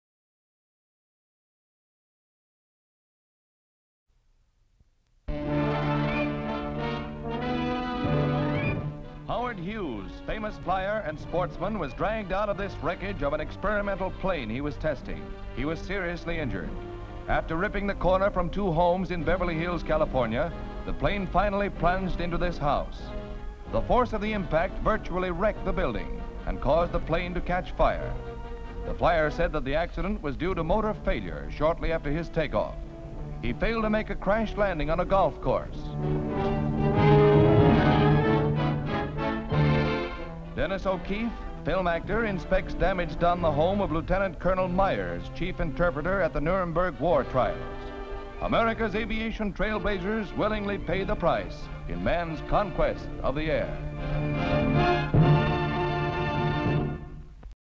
Aviator Howard Hughes survives an experimental plane crash destroying two houses ...HD Stock Footage
Aviator Howard Hughes survives an experimental plane crash destroying two houses in Beverly Hills, California.

Aviator Howard Hughes survives a crash in Beverly Hills, California. Howard Hughes poses. Damaged houses where the experimental plane of Hughes crashed and wreckage of the plane. A lieutenant walks up the stairs inside a damaged house. Location: Beverly Hills California. Date: July 11, 1946.

57,000+ broadcast-quality historic clips for immediate download.